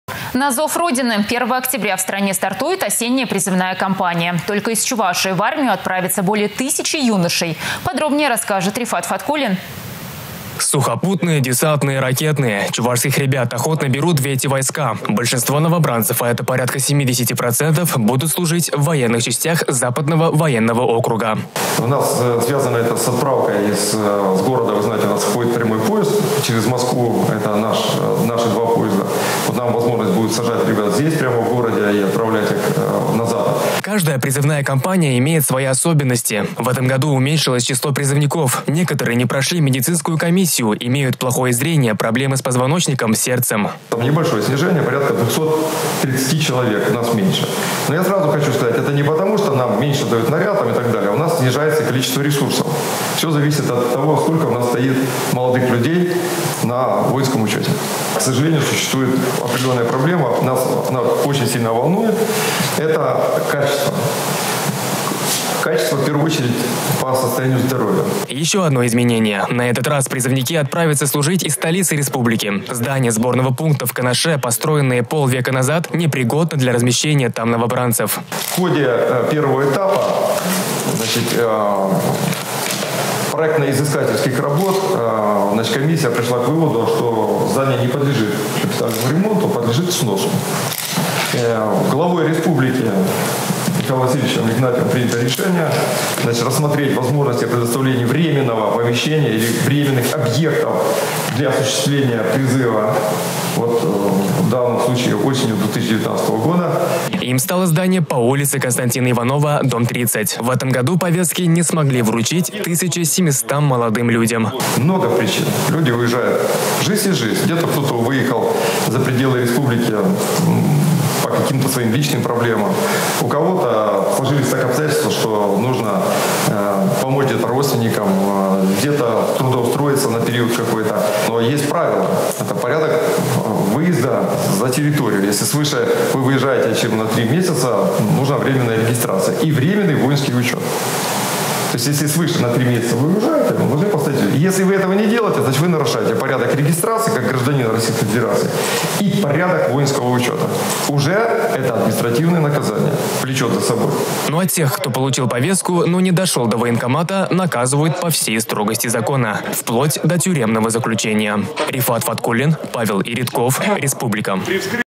На зов Родины
На зов Родины. 1 октября в стране стартует осенняя призывная кампания. Только из Чувашии в армию отправятся  более тысячи юношей. Подробнее расскажет Рифат Фаткуллин.